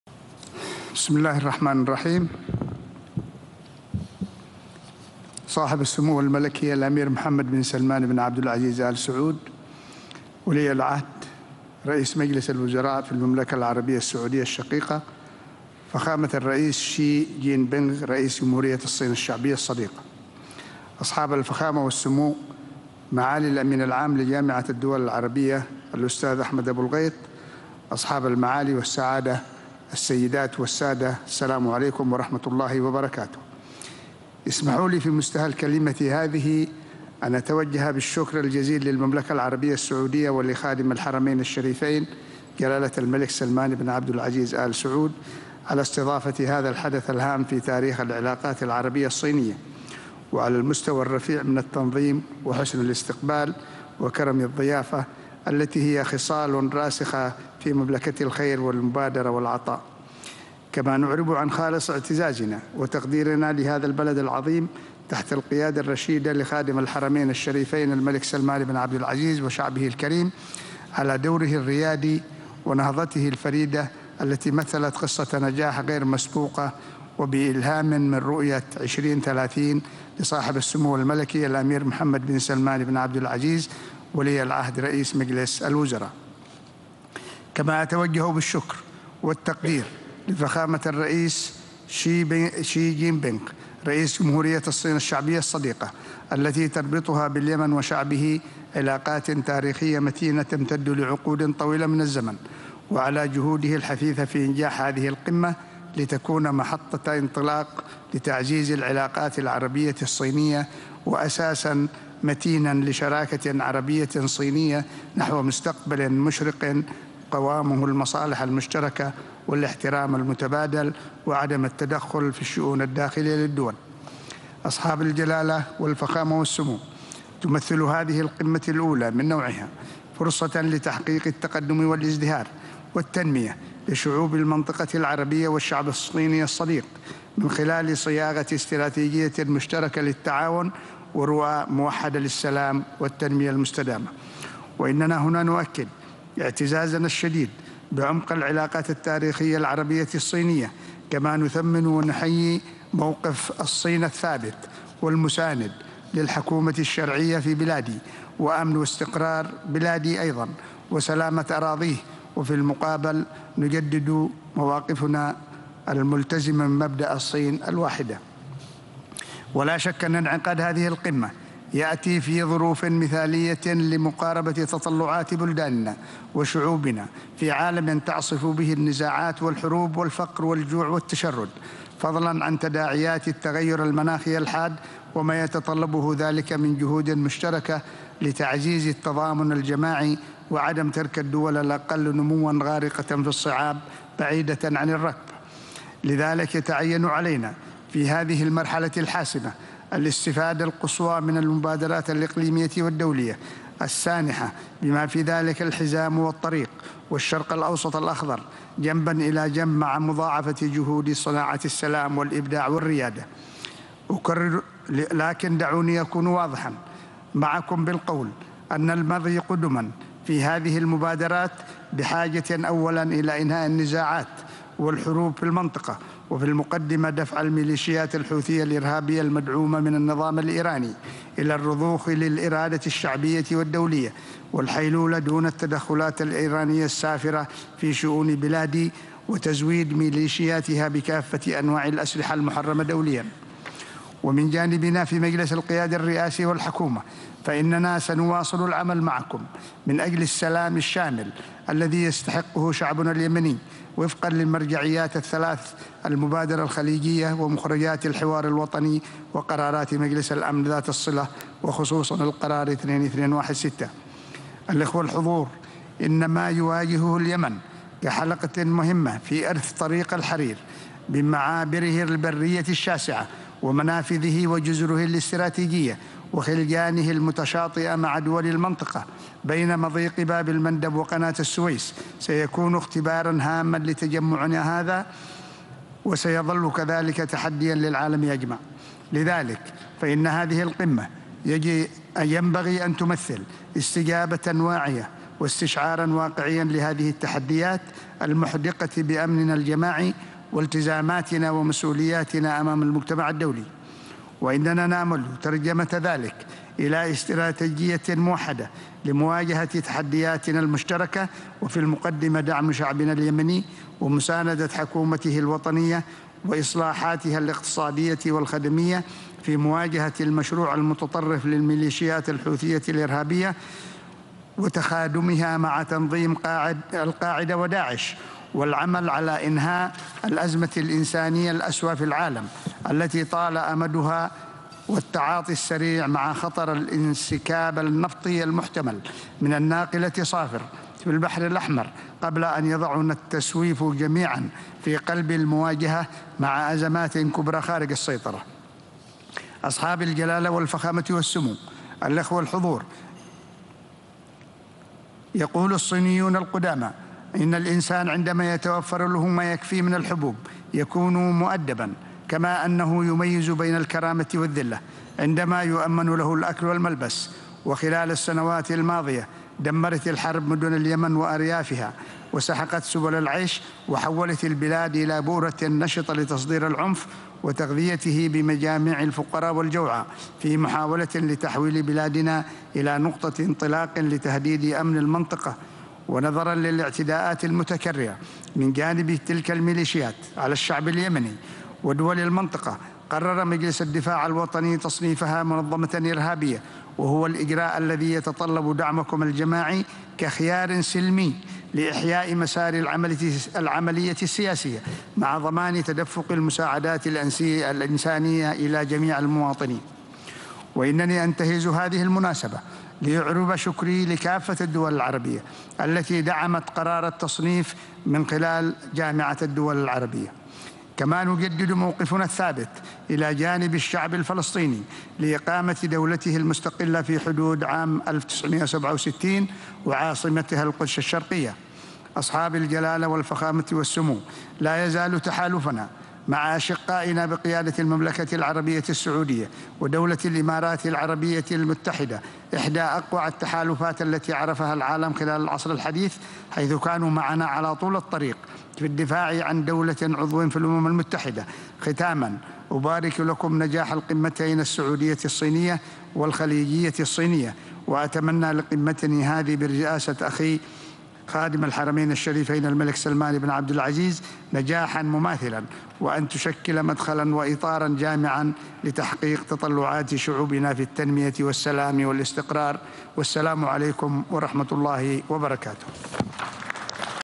كلمة رئيس مجلس القيادة الرئاسي اليمني رشاد العليمي أمام قمة الرياض العربية الصينية للتعاون والتنمية
تغطية خاصة |كلمة رئيس مجلس القيادة الرئاسي اليمني رشاد العليمي أمام قمة الرياض العربية الصينية للتعاون والتنمية